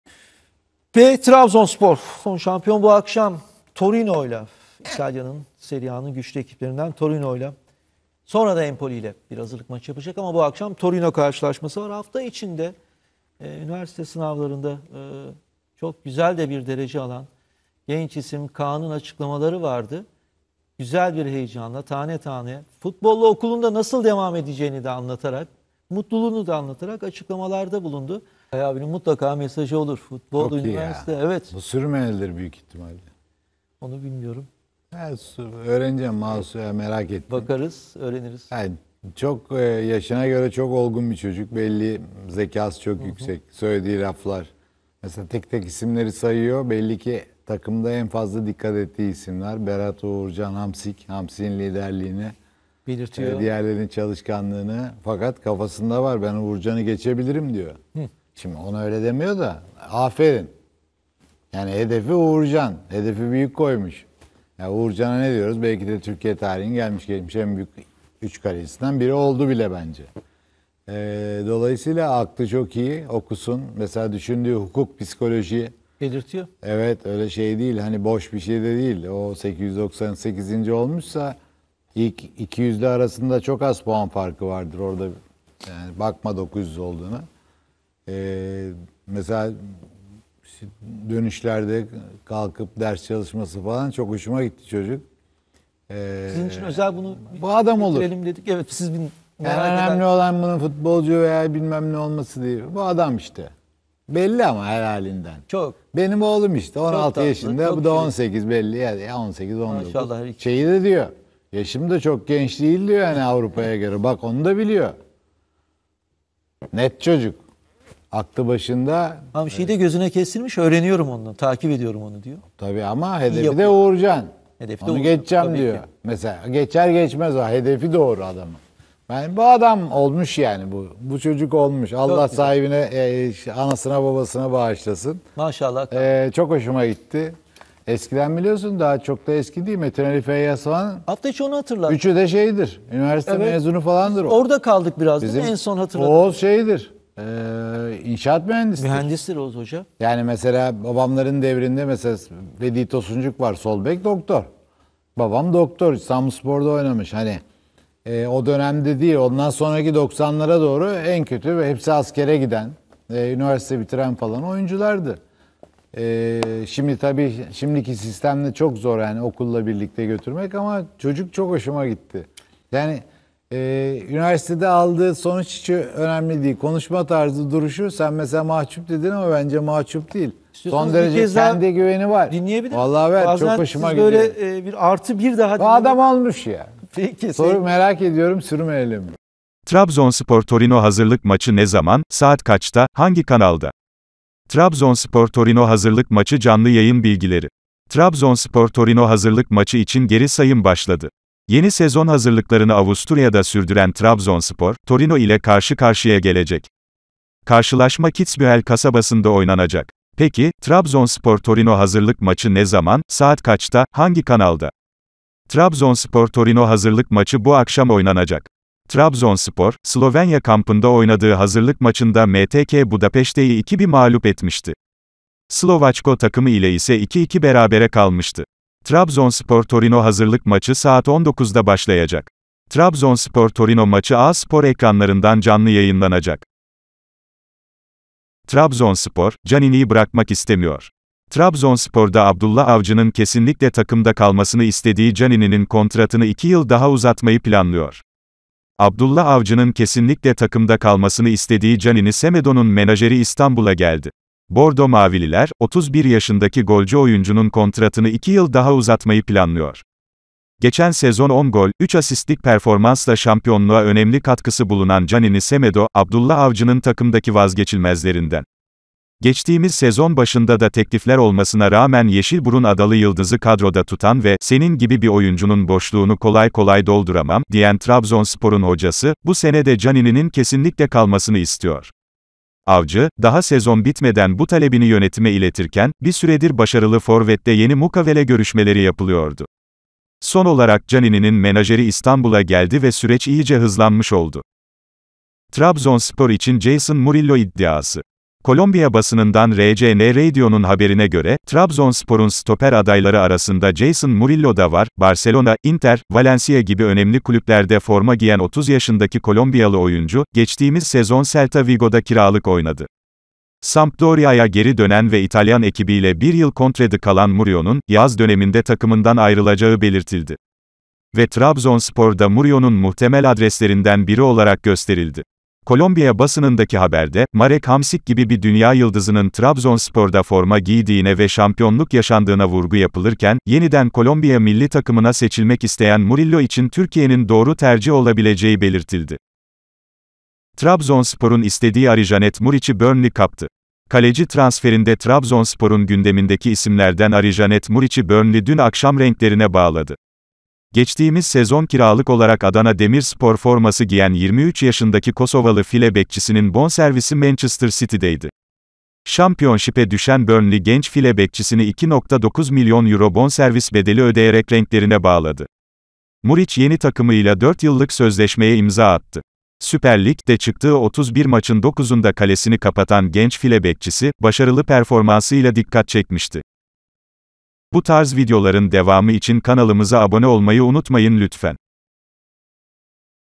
Kağan Moradaoğlu'na Övgü Dolu Sözler !!! Trabzonspor Transfer Haberleri !!!
Trabzonspor'un Avusturya kampı devam ediyor. Trabzonspor'un genç file bekçisi Kağan Moradaoğlu kampta basın mensuplarının sorularını yanıtlarken Kaya Çilingiroğlu da önemli yorumlarda bulundu. Kaya Çilingiroğlu Kağan için oğlum gibi çok akıllı ve zeki bir çocuk değerlendirmelerinde bulundu. Trabzonspor'da transfer çalışmaları da tüm hızıyla sürüyor. Trabzonspor stoper transferinde yeni alternatif isimler ile de görüşüyor. Ayrıntılar videomuzda. Keyifli seyirler...